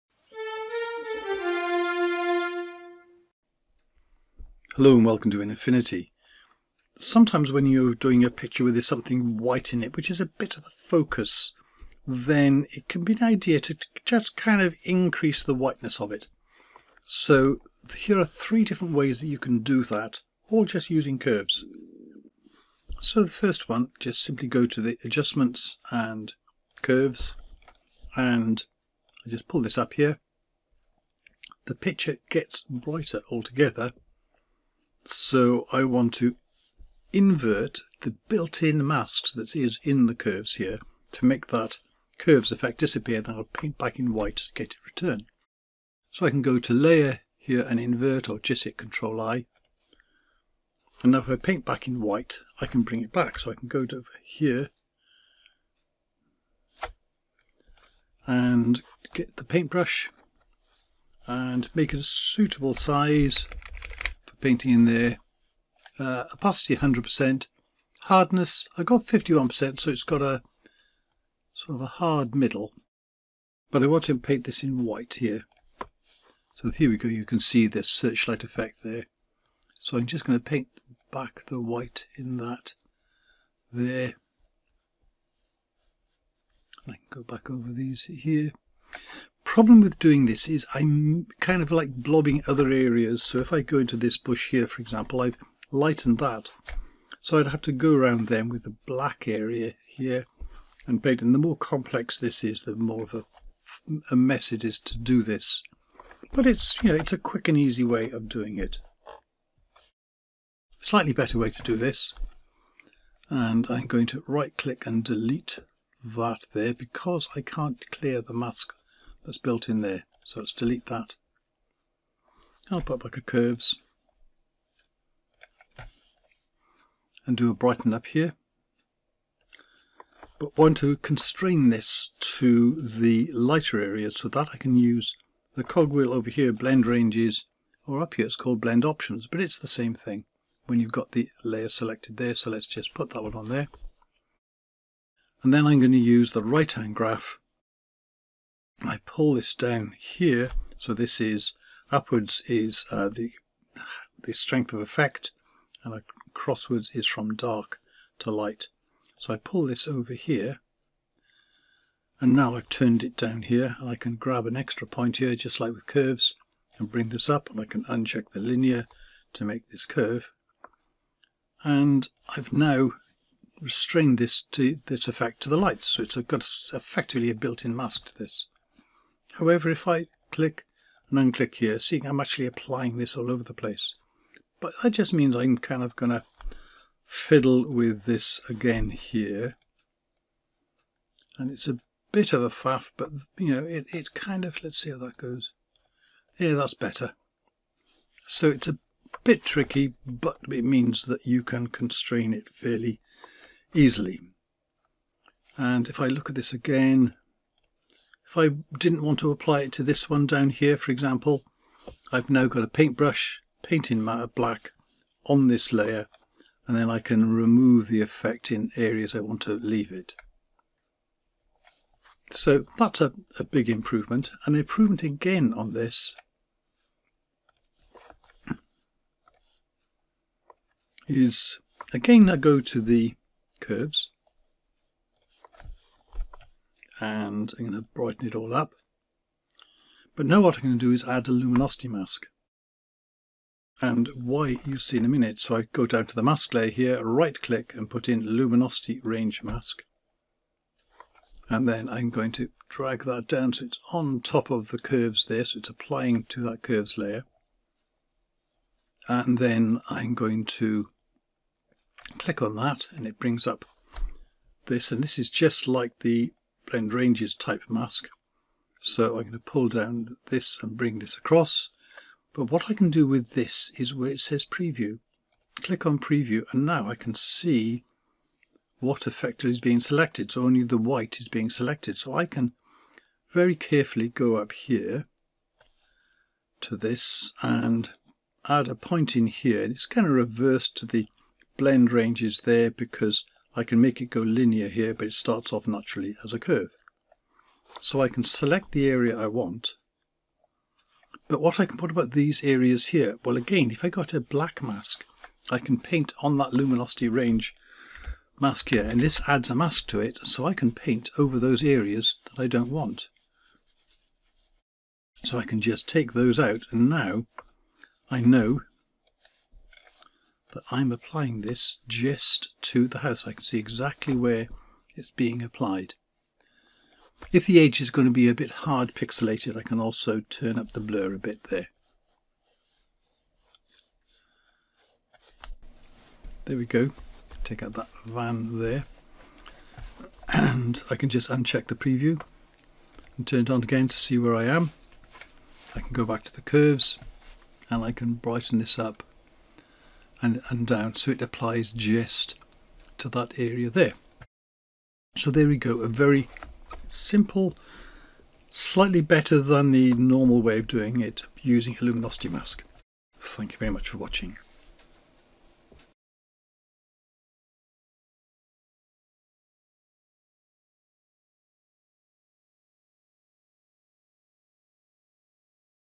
Three Ways to use Curves to Enhance Whites
Here are three different ways you can boost whites to create a stronger 'pull', from quick-and-easy to more powerful (and still pretty easy). We use Curves here as a well-known adjustment while doing this. You can of course you all kinds of other adjustment or filter with varying Blend Modes, for example a Fill Layer, Selective Colour or simple Brightness.